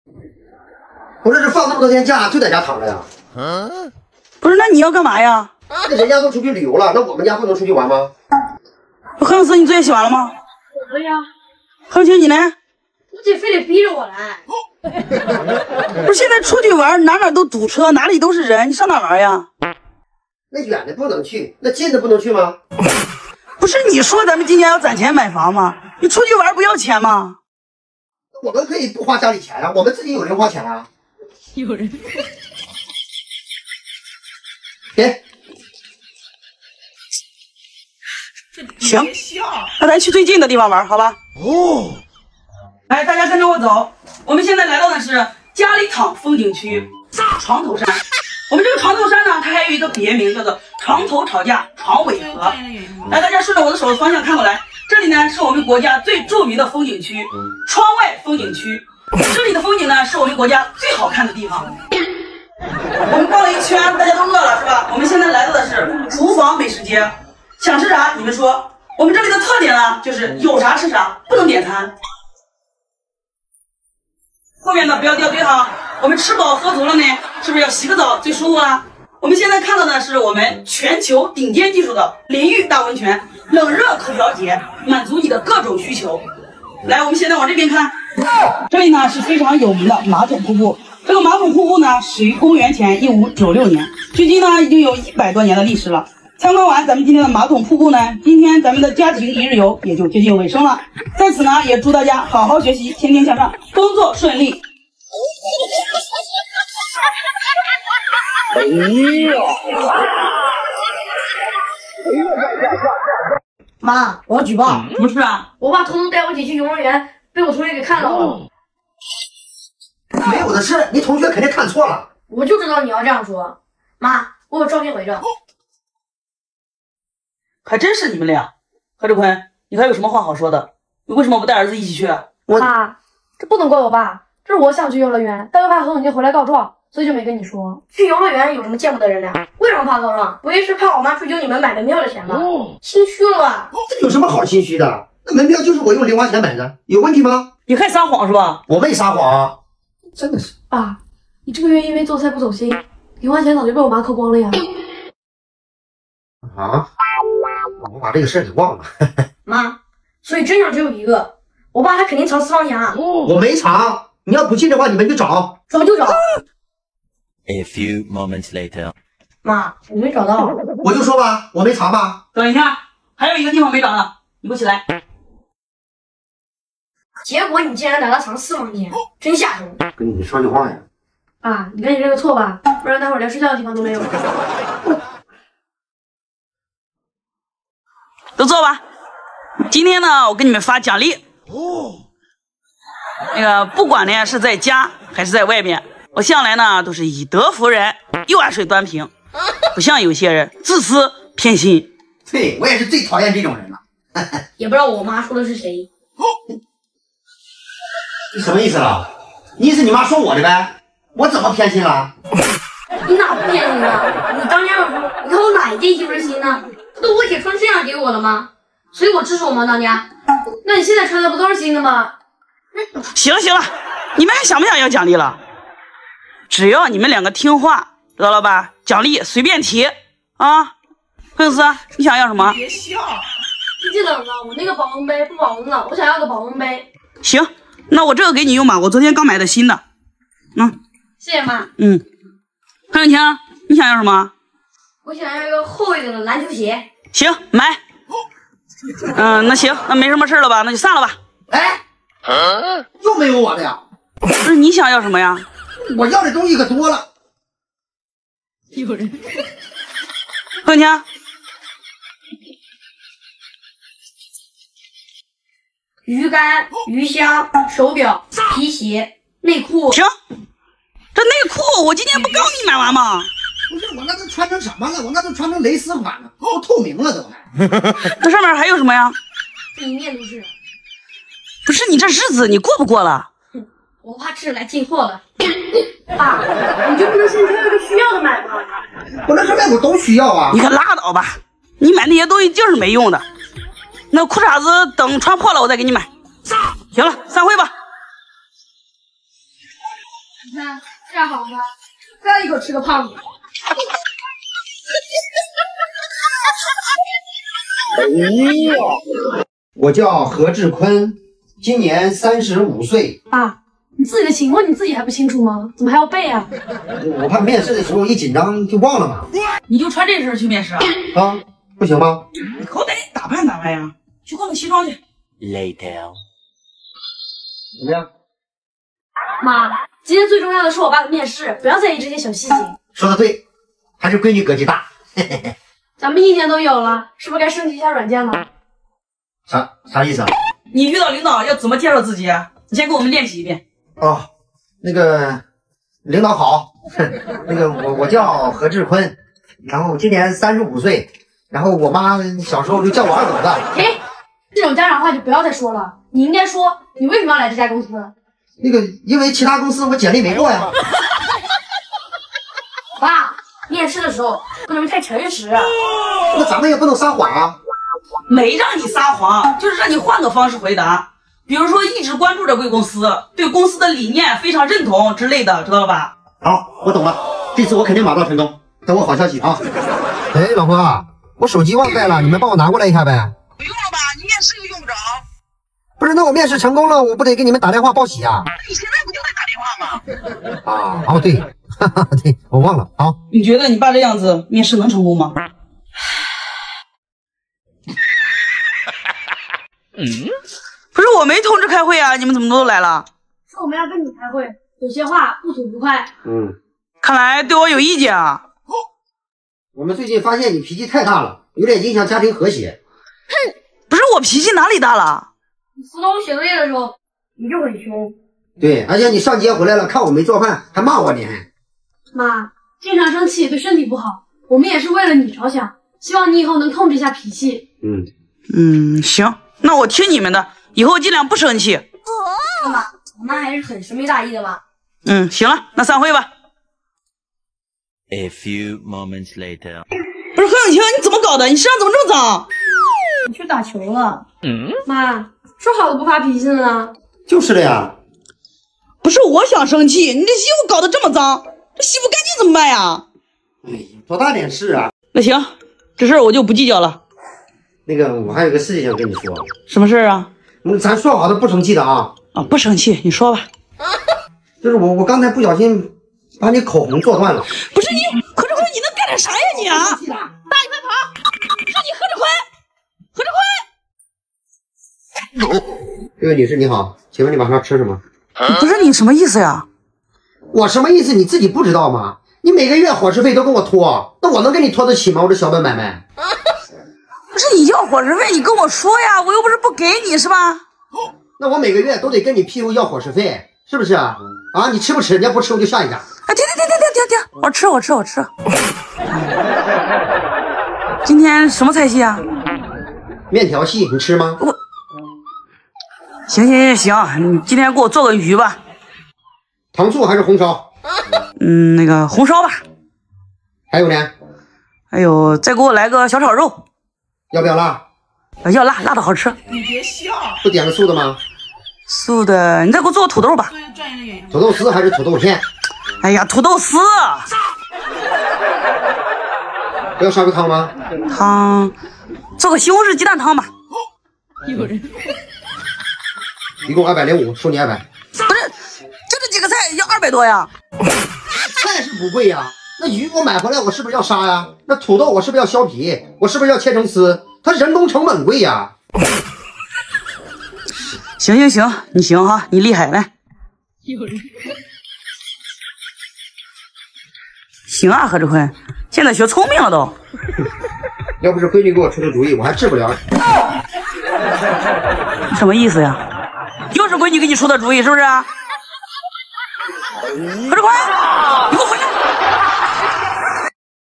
老爸放假想出去玩還得和老媽鬥智斗勇，老媽分分鐘帶全家來了“床頭山”，老爸還是天真了!【快樂父子】
“ 我們製作的影片完全符合平台的使用政策，絕對沒有任何不良內容。”